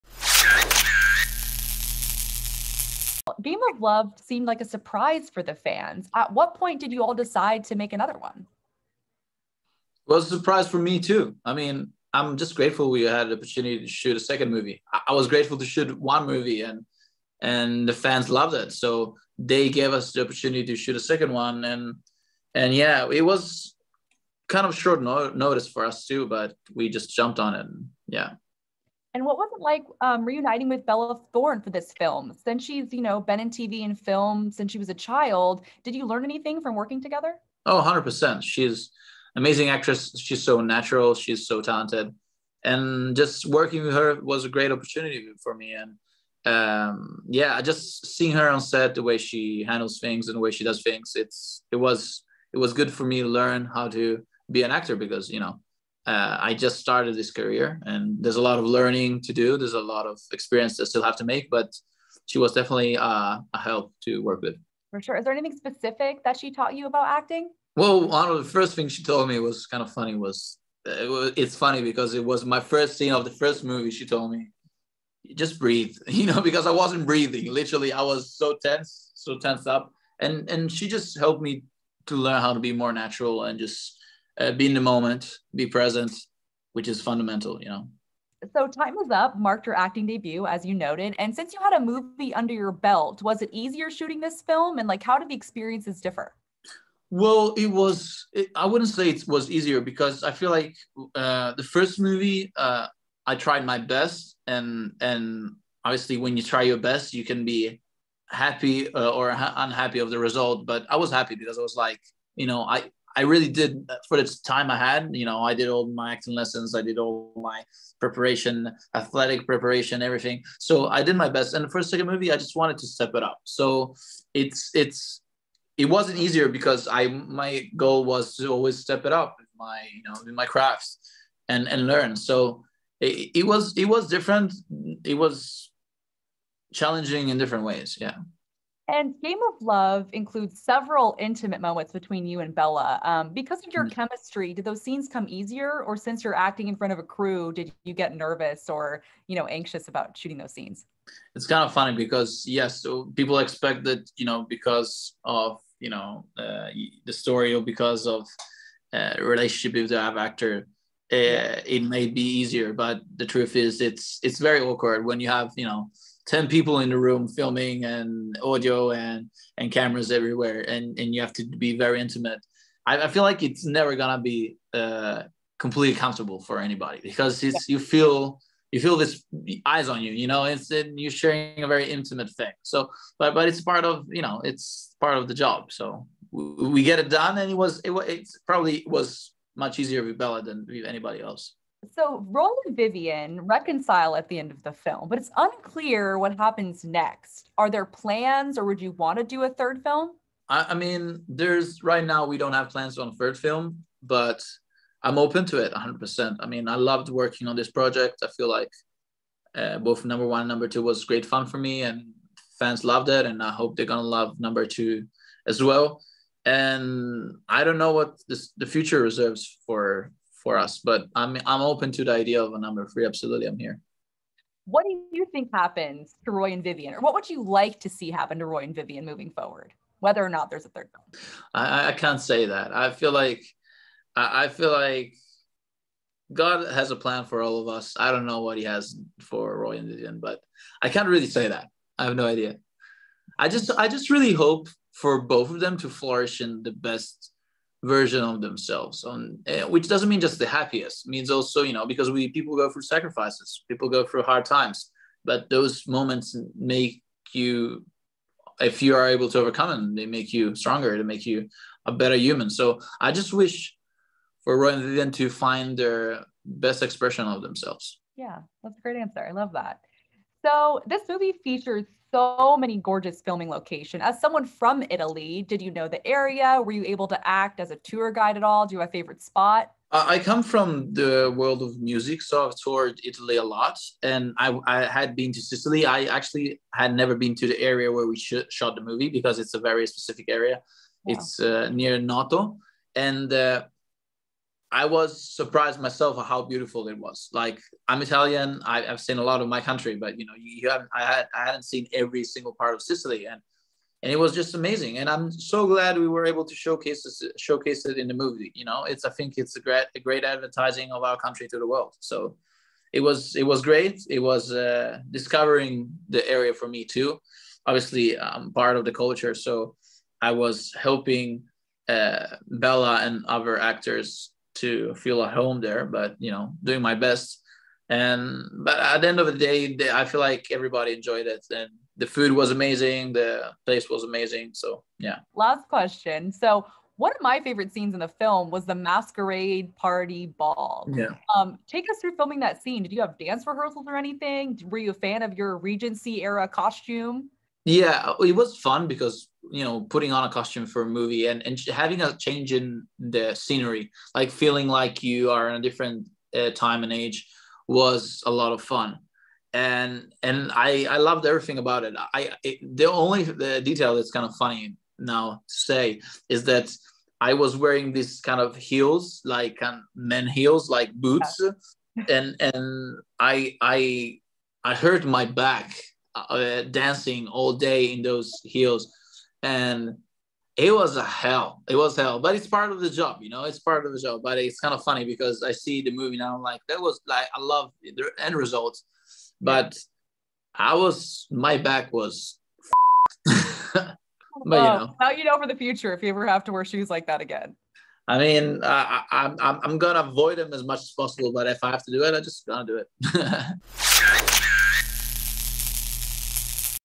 Game of Love's Benjamin Mascolo On Working with Bella Thorne, Filming Intimate Scenes | toofab
The actor opens up about working with his now-ex Bella Thorne on the romantic drama "Game of Love," which is a sequel to the 2021 film, "Time Is Up." Mascolo gets candid about his experience shooting intimate scenes with Thorne, and reveals what he's learned from her about acting. He also shares what it was like shooting in Italy and whether or not he'd be open to doing a third film.